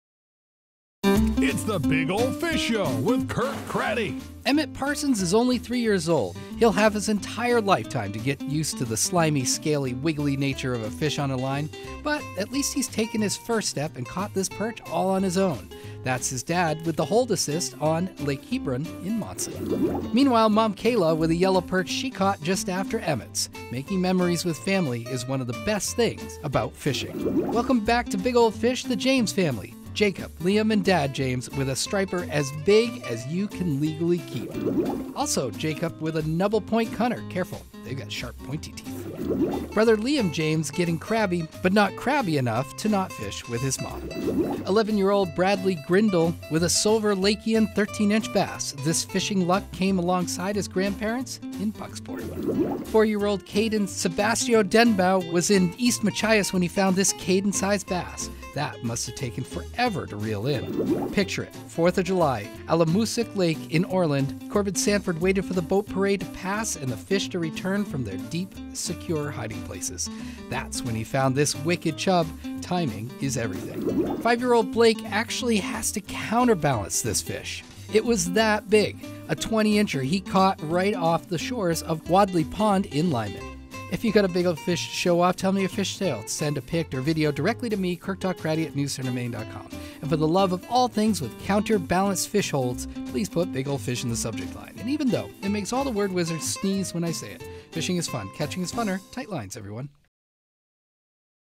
Big Ol' Fish: The Counterbalanced Fish Hold
A fish so big he had to counterbalance himself!